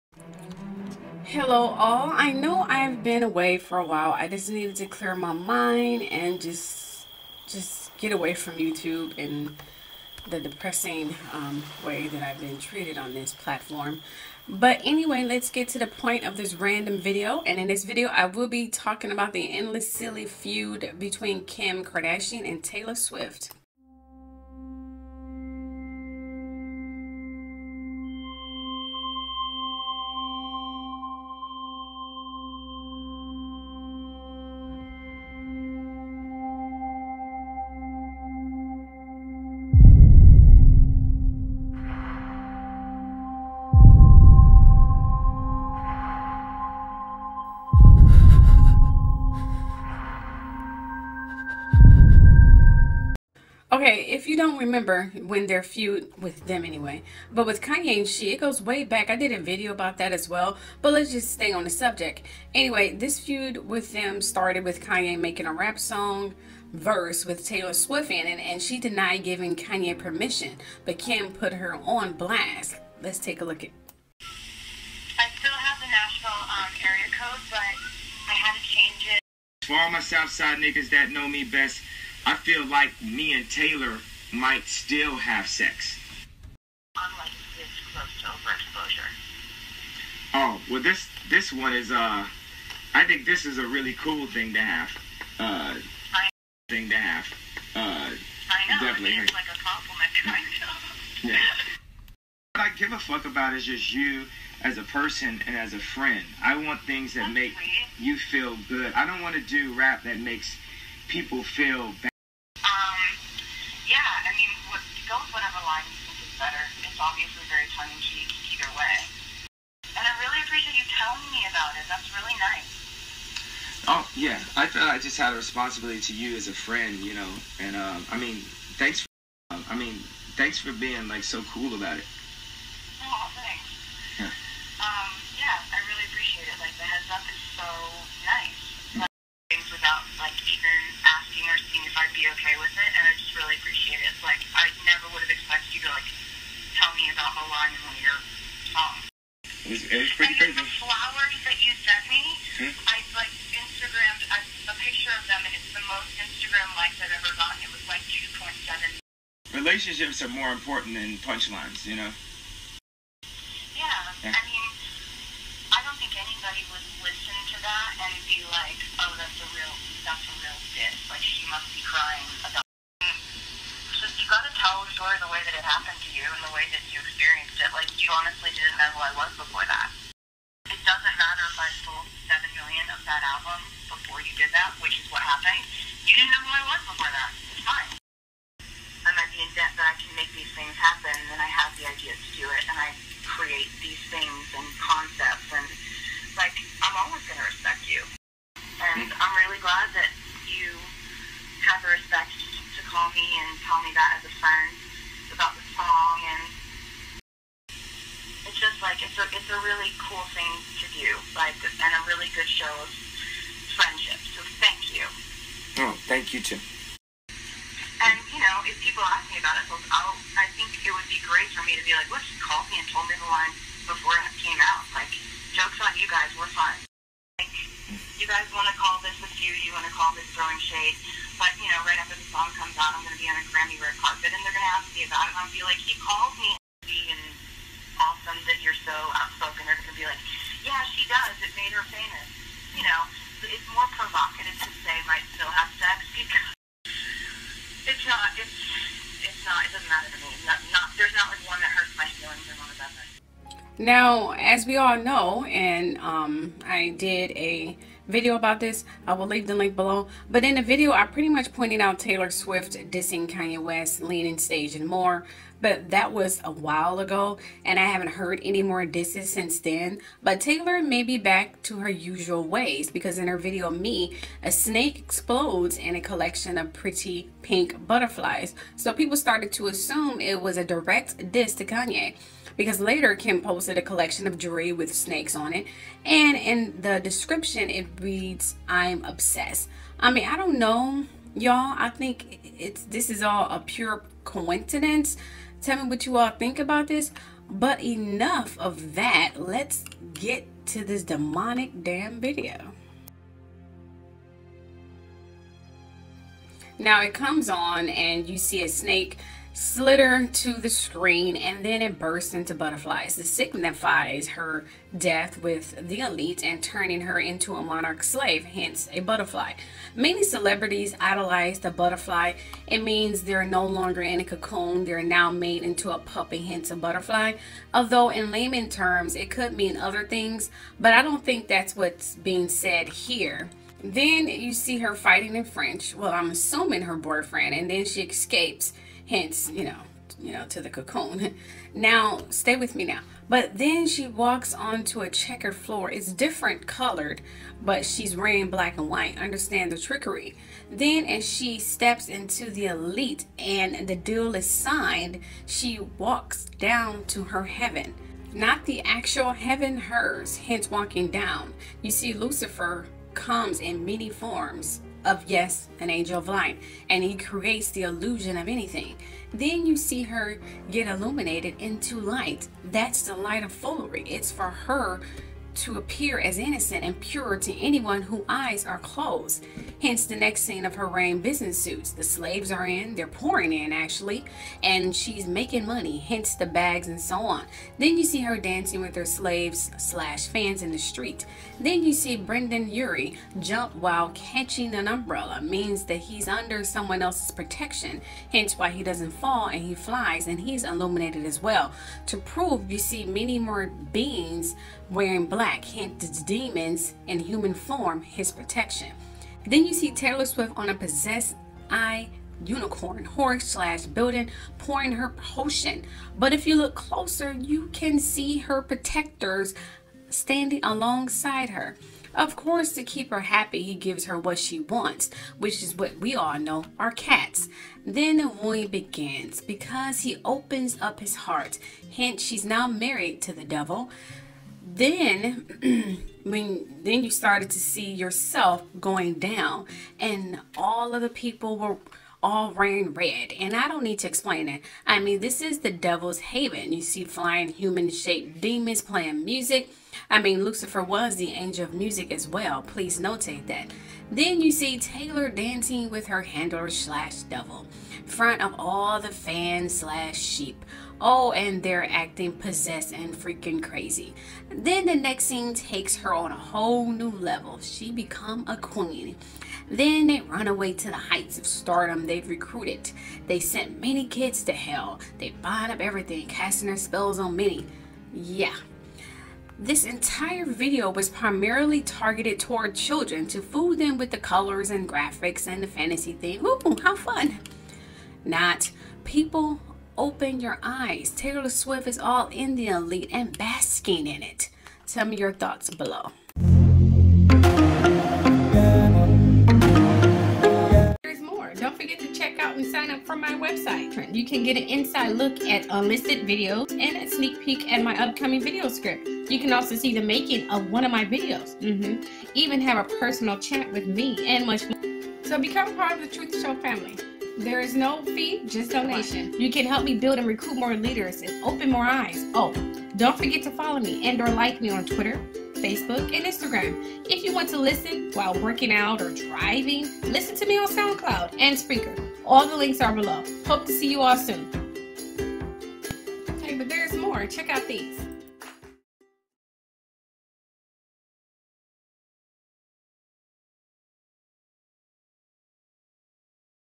Kim Kardashian and Taylor Swift are Back at it Again!! ALL THE TEA and MORE!!
📣🚨RANDOM VIDEO ALERT‼️Here we are with a random video, and in this video I will be talking about the endless silly feud between KimKardashian and TaylorSwift and More.

Videos Used/Mentioned are Listed Below...